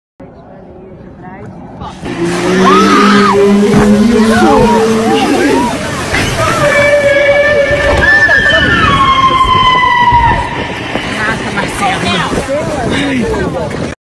grey whale jump out near to a canoe
You will never believe what he will find.
biggest whale
biggest whale ever
biggest whale in the ocean
biggest whale in the world
biggest whales